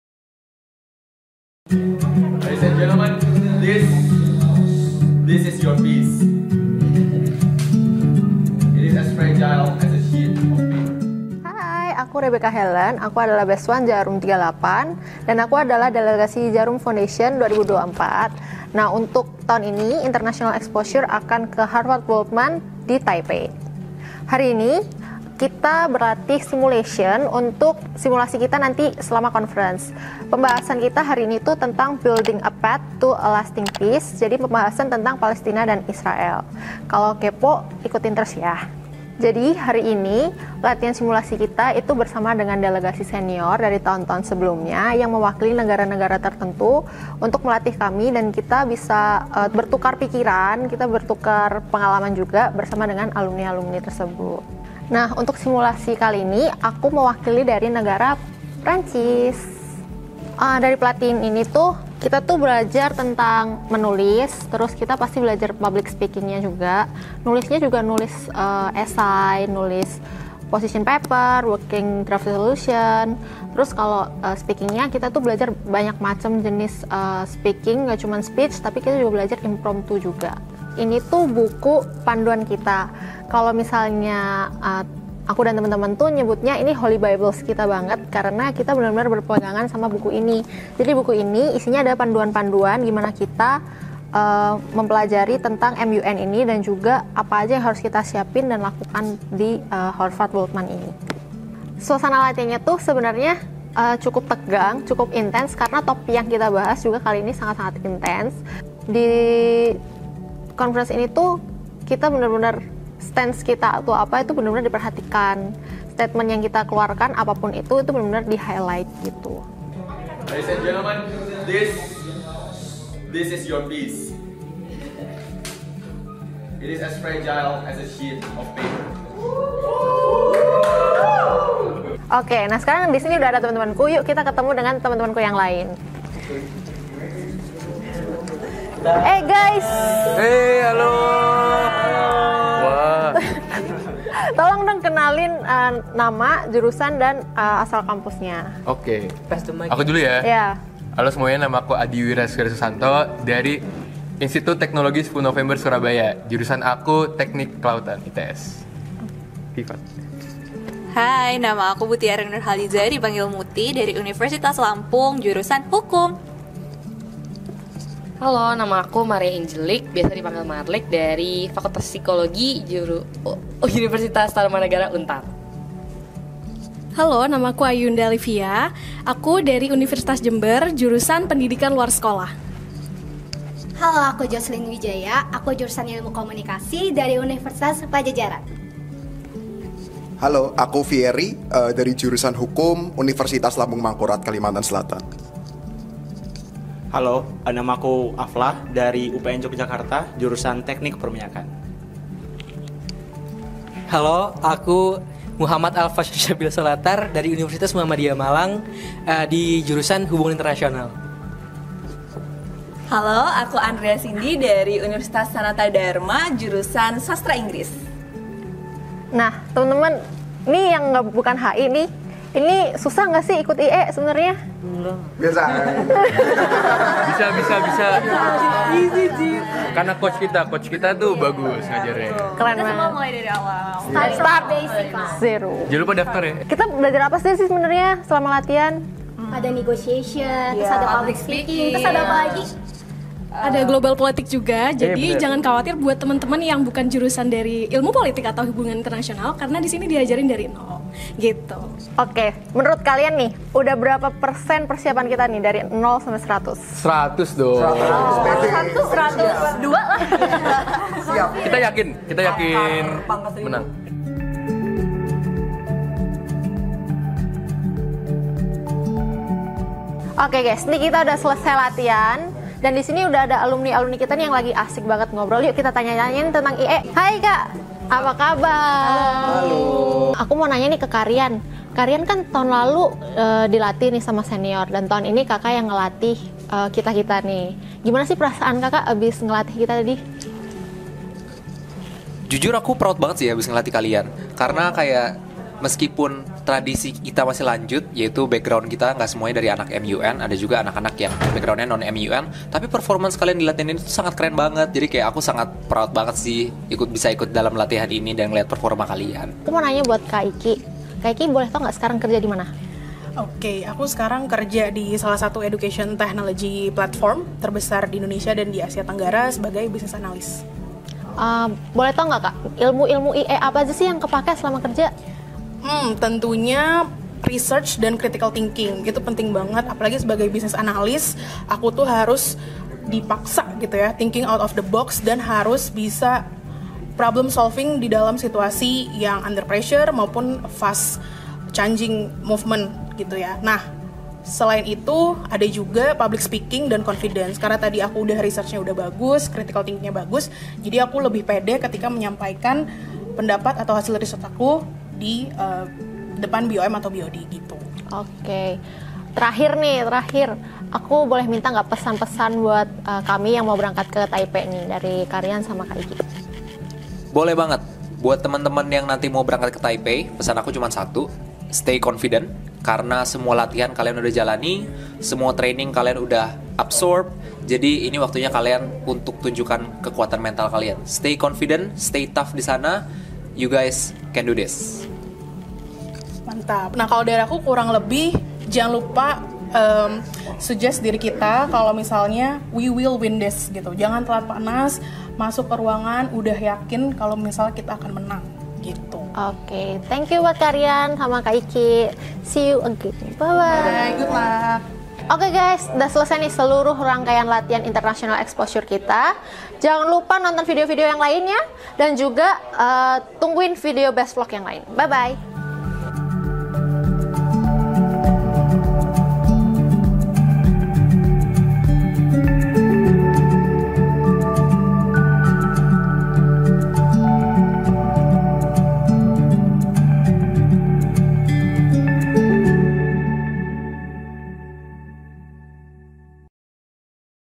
Latihan Rasa MUN Beneran! - International Exposure | BesVlog 005
Sebelum berangkat ke Harvard WorldMUN 2024 di Taipei, Taiwan, Delegasi Djarum Foundation mendapat serangkaian pelatihan intensif.
Dalam salah satu sesi pelatihan, para delegasi  berkesempatan melakukan simulation preparation bersama delegasi-delegasi dari angkatan sebelumnya. Melalui pelatihan ini, para delegasi bisa merasakan suasana dan dinamika sidang MUN.